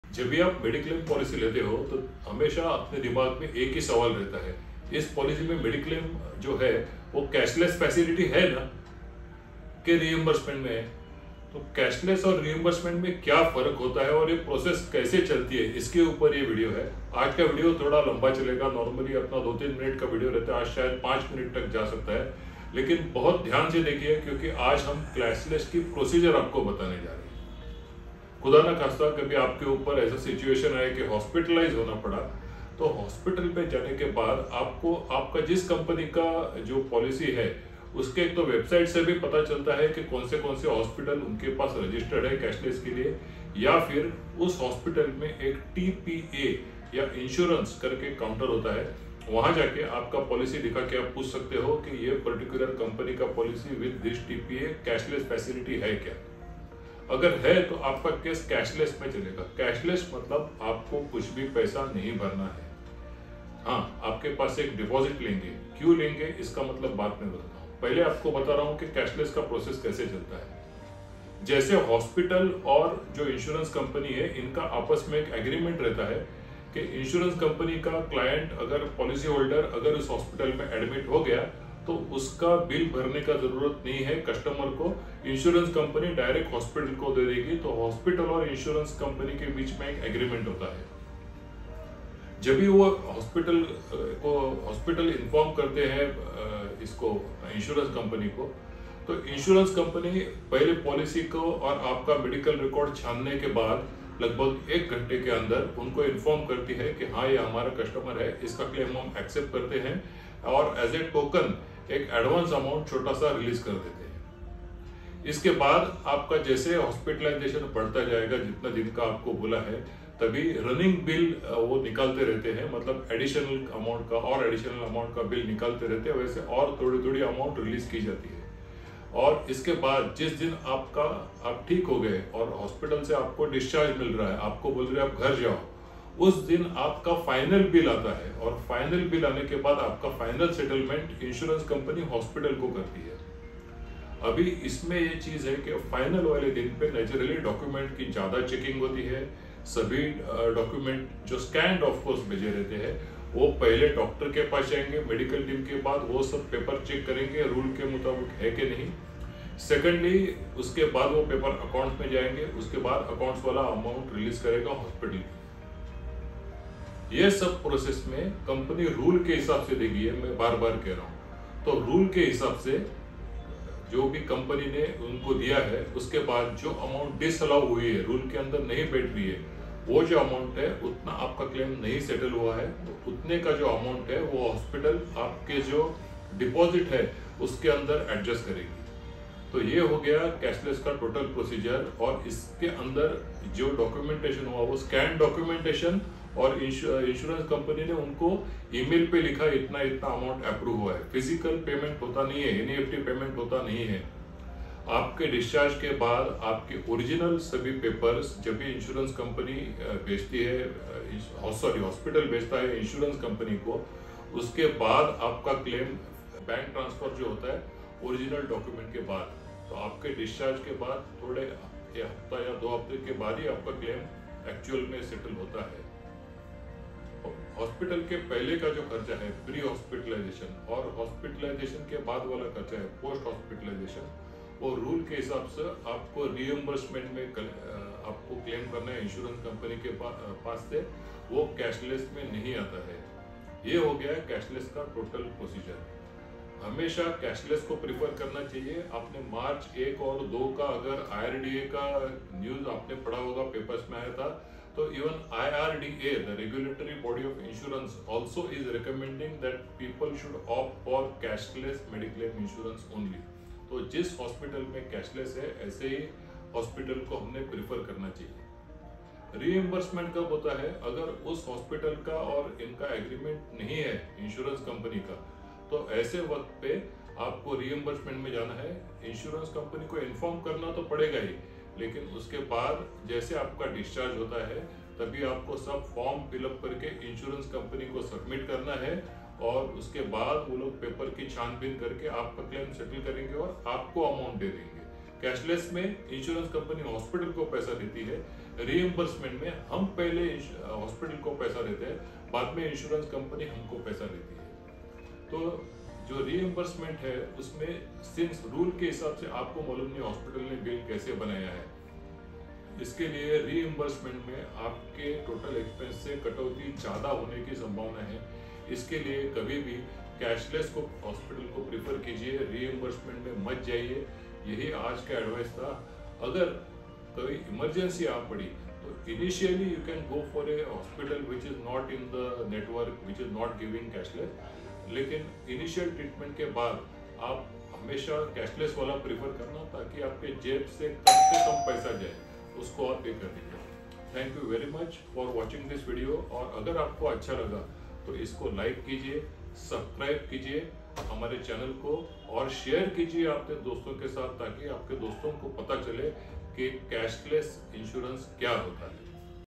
Cashless Mediclaim Procedure | Cashless Mediclaim Process | How to use Cashless Mediclaim
Whenever someone buys a Mediclaim policy, the first question always  is does it offer Cashless facility?

Cashless if a facility offered by Health Insurance Company only when Hospital has signed agreement with insurance Company.

Understand how the procedure takes place when policyholder is hospitalised.

Decide whether to use cashless facility or to go for reimbursement.